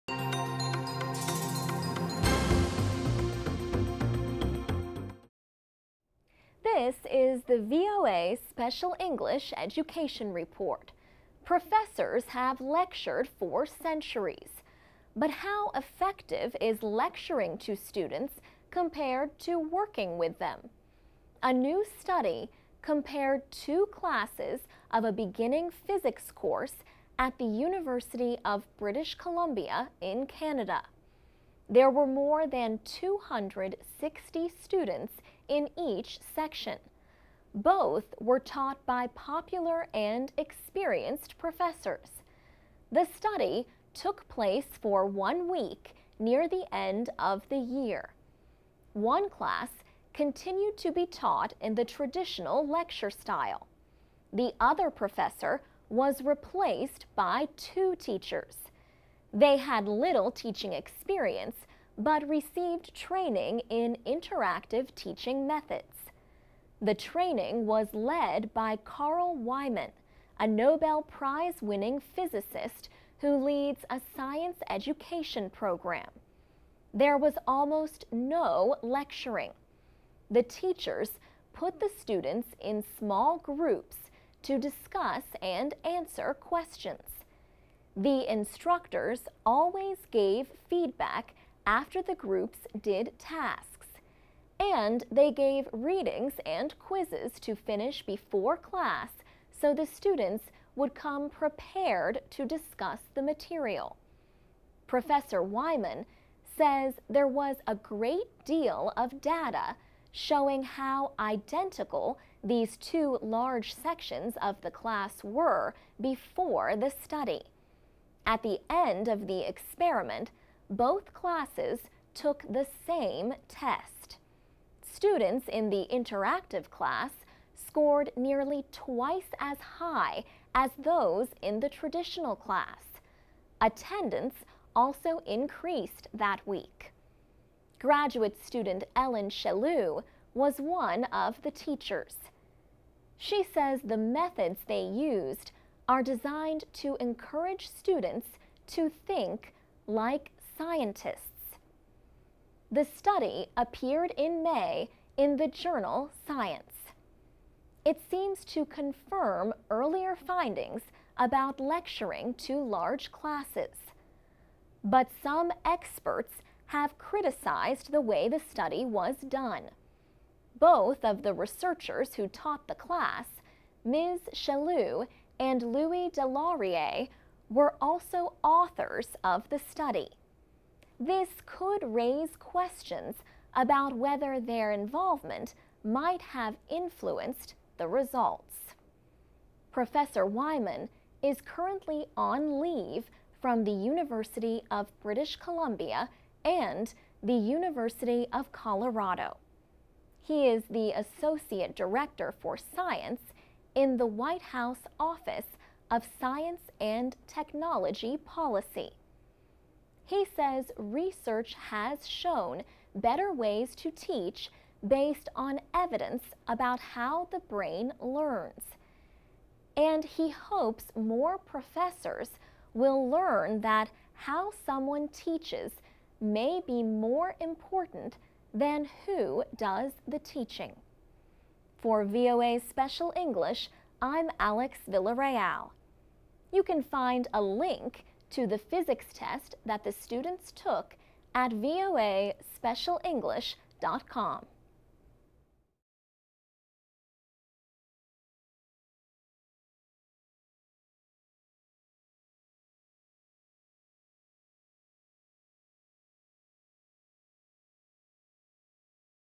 Phát âm chuẩn cùng VOA - Anh ngữ đặc biệt 495 education (VOA)
Chương trình học tiếng Anh của VOA: Special English Education Report - Interactive Teaching vs. Lectures. Xin hãy vào để xem các bài kế tiếp.

This is the VOA Special English Education Report.

Professors have lectured for centuries. But how effective is lecturing to students compared to working with them? A new study compared two classes of a beginning physics course at the University of British Columbia in Canada. There were more than two hundred sixty students in each section. Both were taught by popular and experienced professors.

The study took place for one week near the end of the year. One class continued to be taught in the traditional lecture style. The other professor was replaced by two teachers. They had little teaching experience but received training in interactive teaching methods. The training was led by Carl Wieman, a Nobel Prize-winning physicist who leads a science education program.

There was almost no lecturing. The teachers put the students in small groups to discuss and answer questions. The instructors always gave feedback after the groups did tasks. And they gave readings and quizzes to finish before class so the students would come prepared to discuss the material.

Professor Wieman says there was a great deal of data showing how identical these two large sections of the class were before the study. At the end of the experiment, both classes took the same test. Students in the interactive class scored nearly twice as high as those in the traditional class. Attendance also increased that week.

Graduate student Ellen Schelew was one of the teachers. She says the methods they used are designed to encourage students to think like scientists.

The study appeared in May in the journal Science. It seems to confirm earlier findings about lecturing to large classes. But some experts have criticized the way the study was done.

Both of the researchers who taught the class, Ms. Schelew and Louis Deslauriers, were also authors of the study. This could raise questions about whether their involvement might have influenced the results.

Professor Wieman is currently on leave from the University of British Columbia and the University of Colorado. He is the associate director for science in the White House Office of Science and Technology Policy.

He says research has shown better ways to teach based on evidence about how the brain learns. And he hopes more professors will learn that how someone teaches may be more important than who does the teaching.